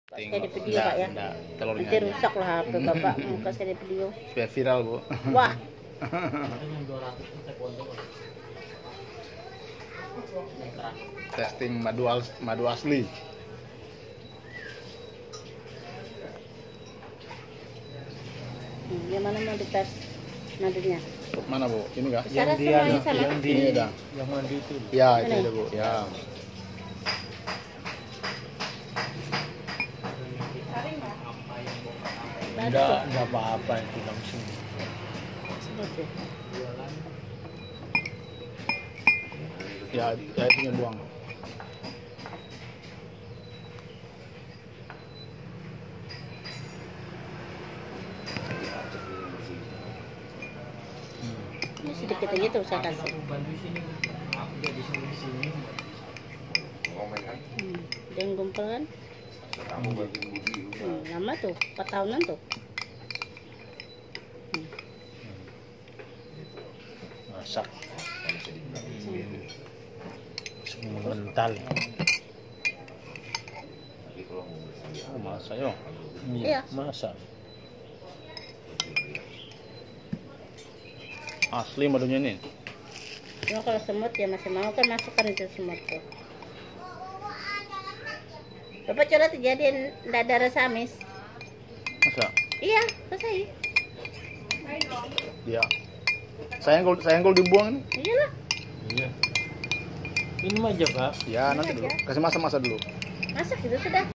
madu murni asli borneo sudah berusia 4 tahun
madu asli murni dari hutan Kalimantan, hutan belantara Kalimantan menghasilkan madu murni yang berkualitas tinggi baik untuk kesehatan